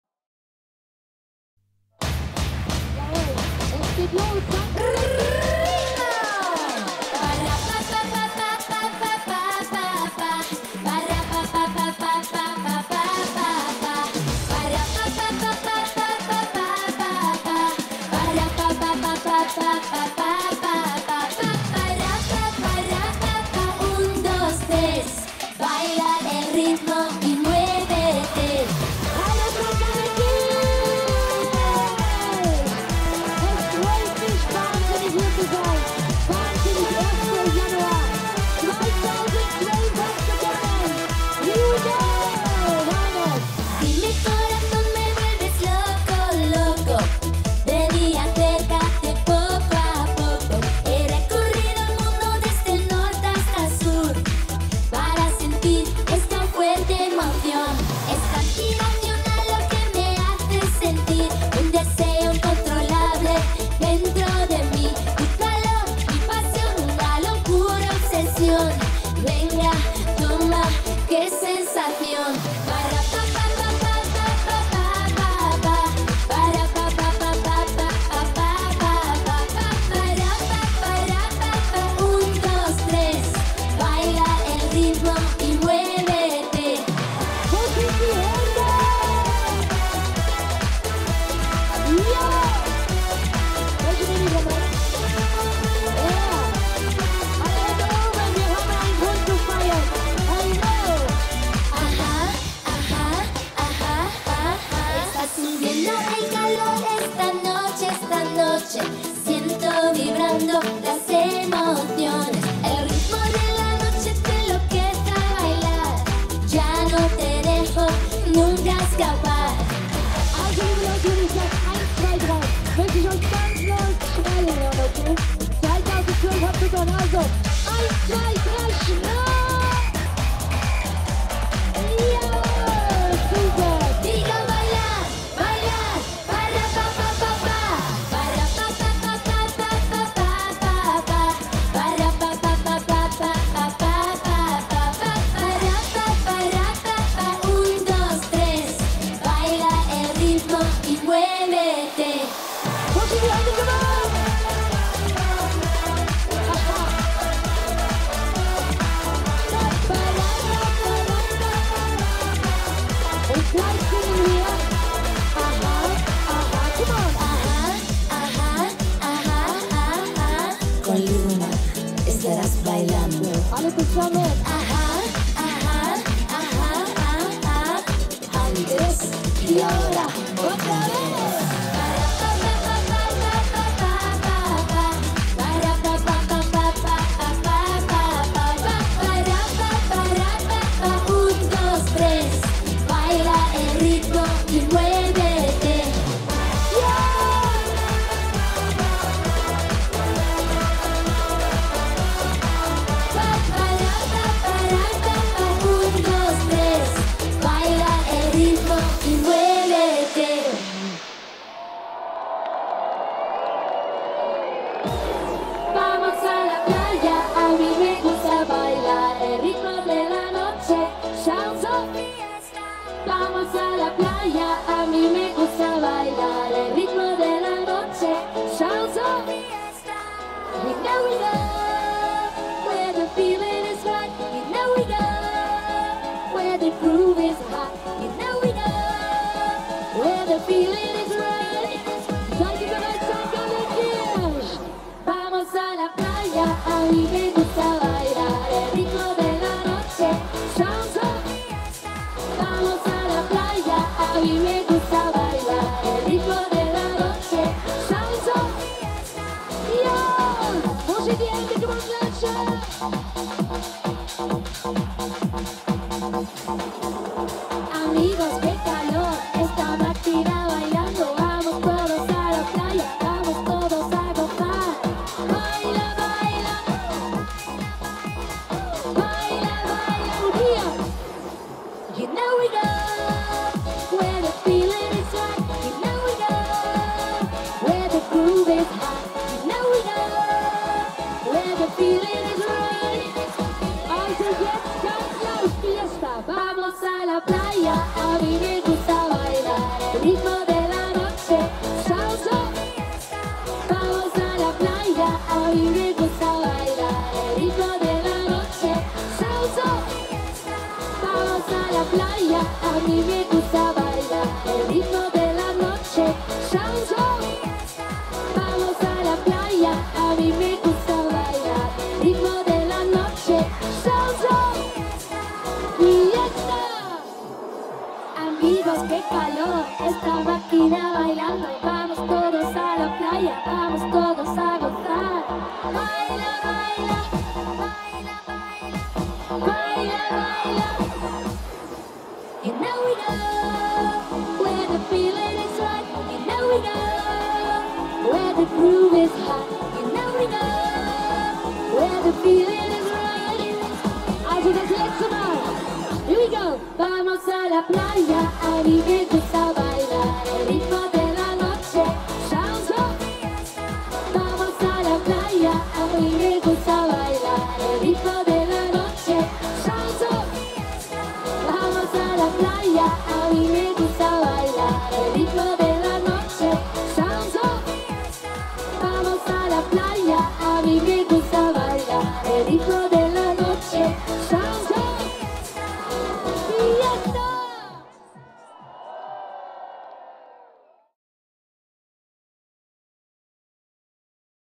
Loona live St. Gallenkirch
Loona live beim Neujahrsfeuerwerk St. Gallenkirch 2012
Kamera: Andre Schnetzer
Schnitt: Frank Schwärzler